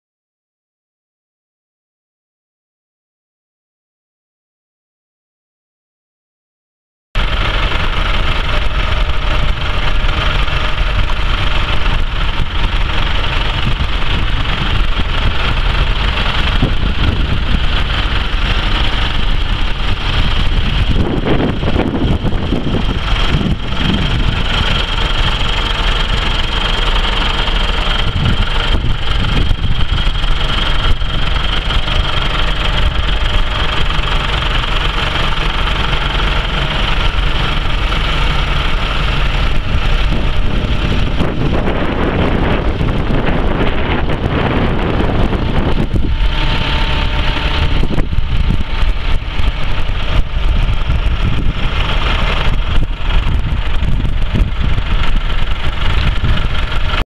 1997 New Holland 7740SL MFWD tractor for sale | sold at auction March 20, 2014
1997 New Holland 7740SL MFWD tractor
5,304 hours on meter
Four cylinder diesel engine
86 HP
Serial PD7436617L12
Synchronized shuttle transmission
12F-12R gears
Locking differential
AC and heat
AM/FM
Rear window protection
Front and rear work lights
GB770 loader
7&#039;W bucket
Bolt-on cutting edge
Bucket lifting eye
Three point
Top link missing
Draw bar
Auxiliary outlet
540 PTO
Rear hydraulic coupler
92" wheelbase
Rear wheel weights
14.9-24 front tires
18.4-34 rear tires
Battery cover missing
Right rear view mirror cracked
Serial 087621B